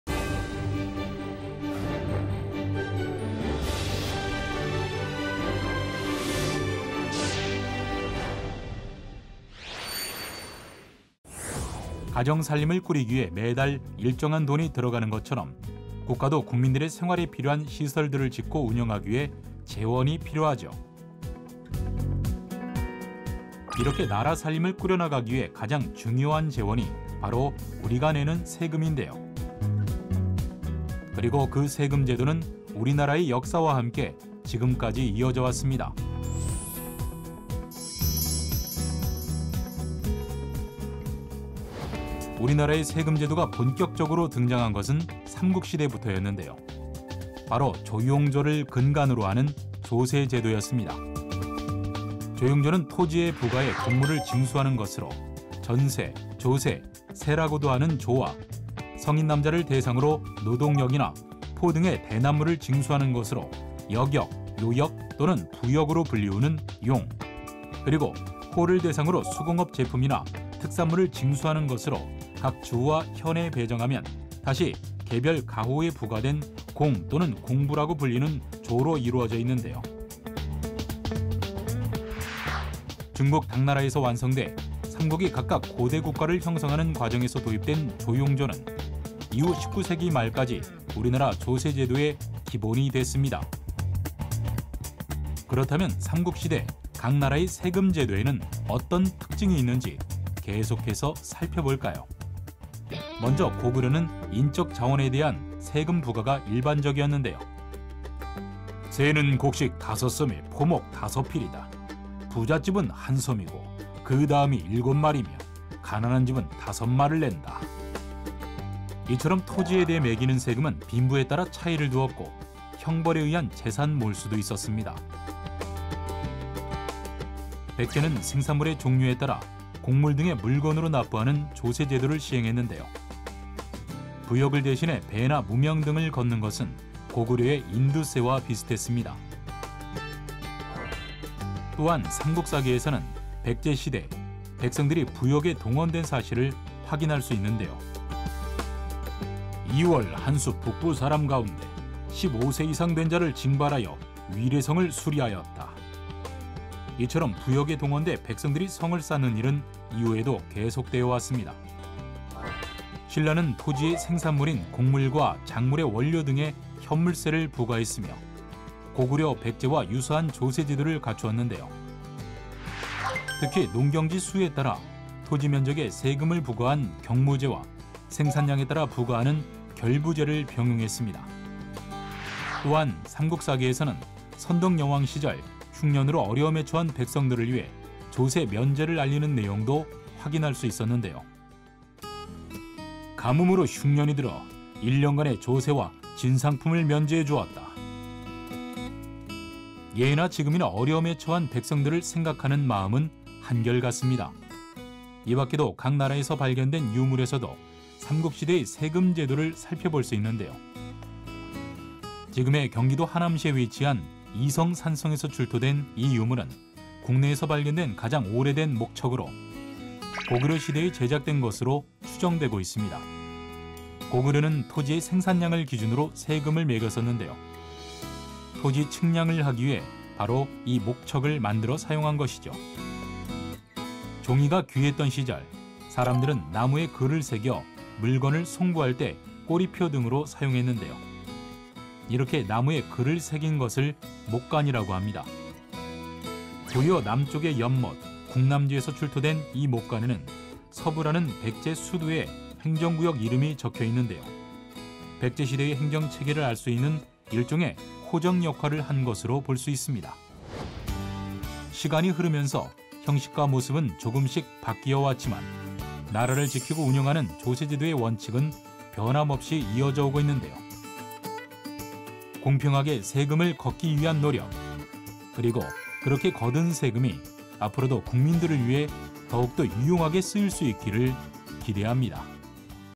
우리나라 조세제도의 시작! 삼국시대 조·용·조(租·庸·調)
여러분에게 세금은 어떤 의미이신가요?
세금은 정부의 재원을 조달하고
소득 양극화를 해소하는 등
자본주의 경제의 문제를 해결하는 주요 수단이기도 한데요.
오늘 역사 속 세금이야기에서는
국민들의 생활에 필요한 시설들을 짓고 운영하는 등
나라 살림을 꾸려나가기 위한 주요 재원인
우리나라 세금제도의 시작에 대해 알아봅니다.